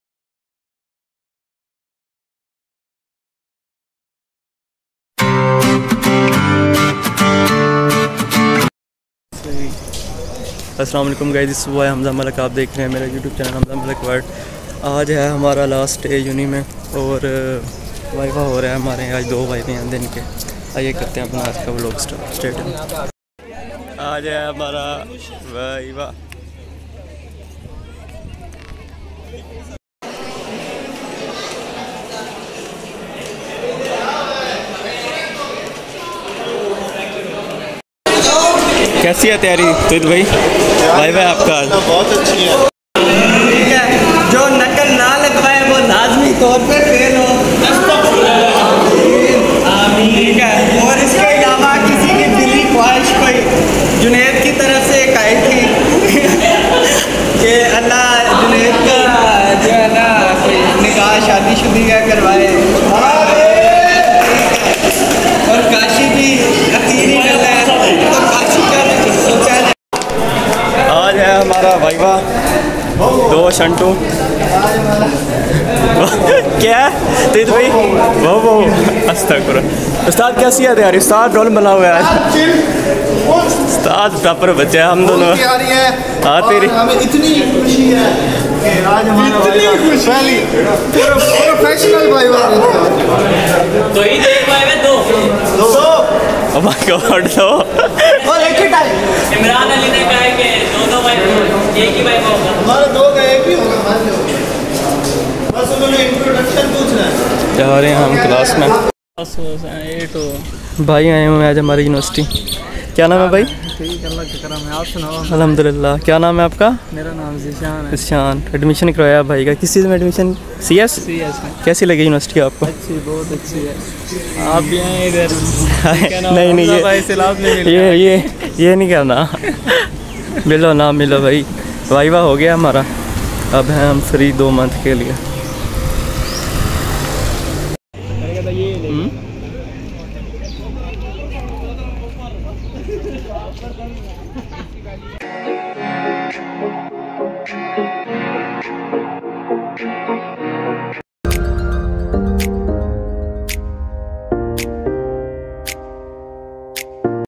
last day in university 🥺| Admission open | Hamza Malik world
AOA today is our last day in university and end of semester.
and also a viva day watch full vlog keep supporting guys
Allah Hafiz ♥️